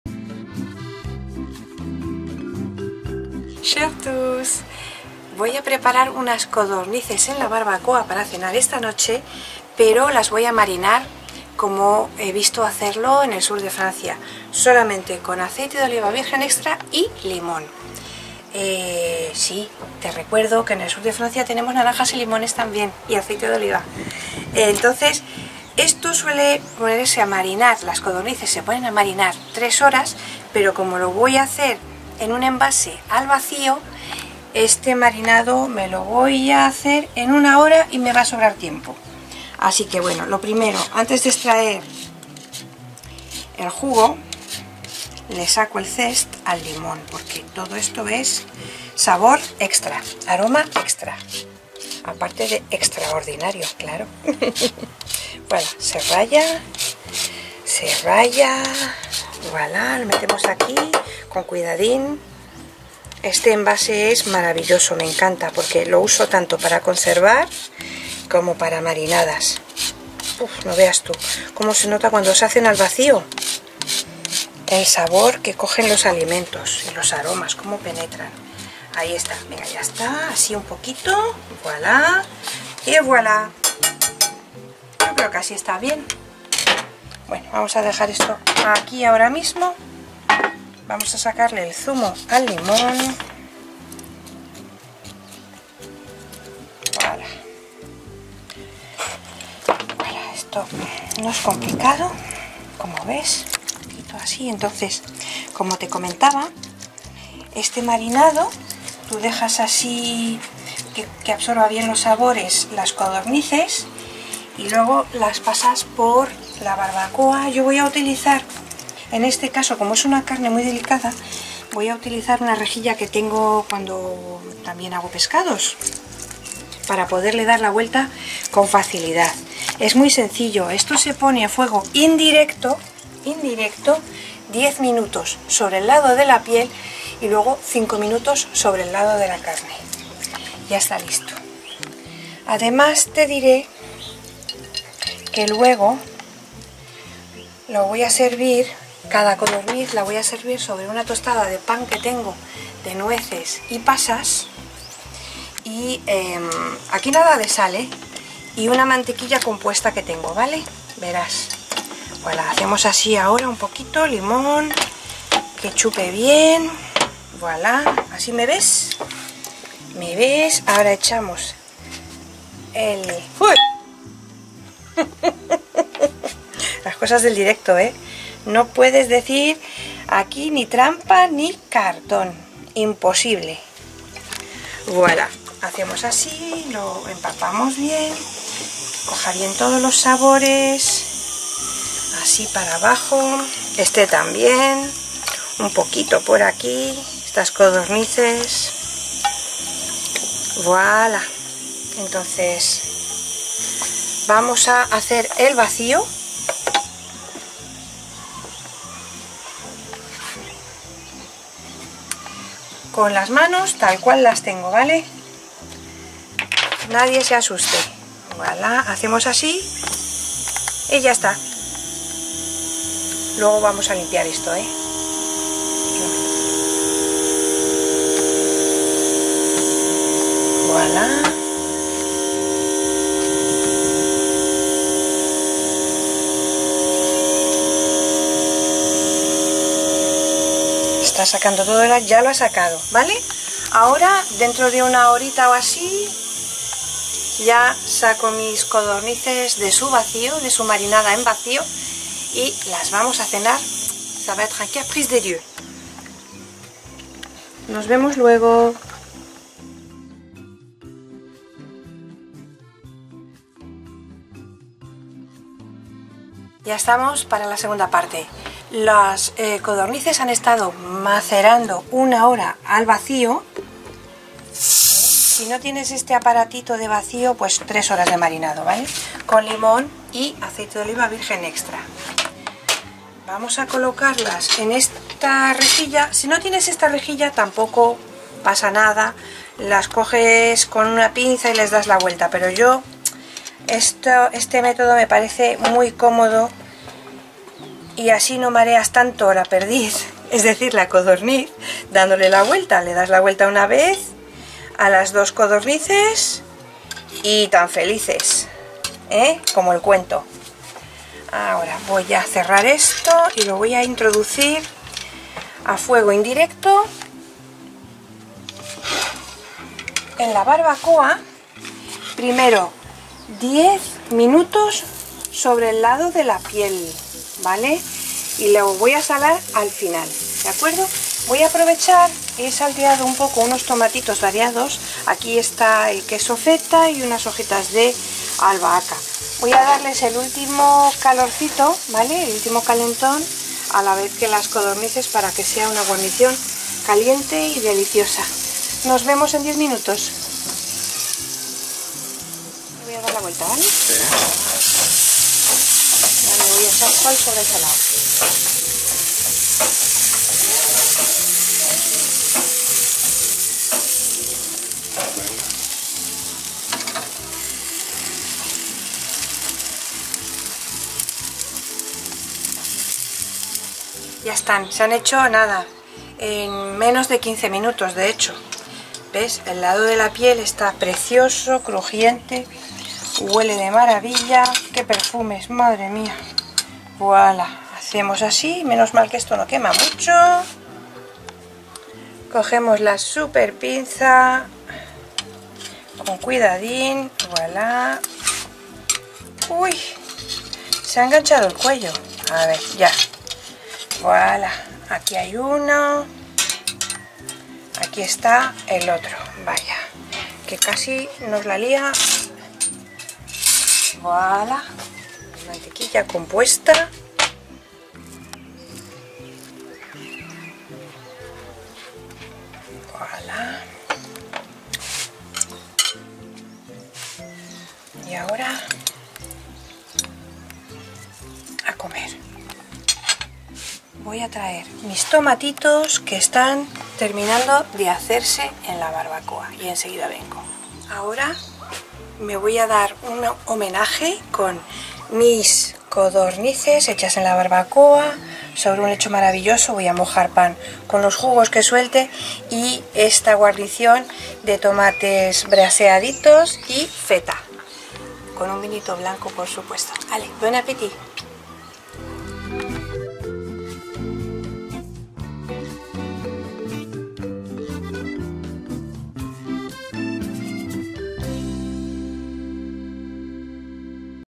Receta francesa de codornices a la parrilla
¡Más fácil imposible y más delicioso aún! Así es esta receta francesa de codornices a la parrilla. Así, tal cual con un marinado típicamente marsellés de limón y aceite de oliva virgen extra.

Ingredientes:
- 2 codornices abiertas en libro (a la sapito o crapaudine)
- el zest y el zumo de un limón
- 1 chorro de aceite de oliva virgen extra DO Sierra Mágina
- sal
- 2 rebanadas de pan de pueblo
- mantequilla con sal o mantequilla compuesta de tu elección.

Recuerde! Pinche en la campanita🔔 para recibir notificaciones de mis próximos videos!
¿Busca más información? No hay problema, haga clic en el enlace del sitio, el traductor de Google está activado.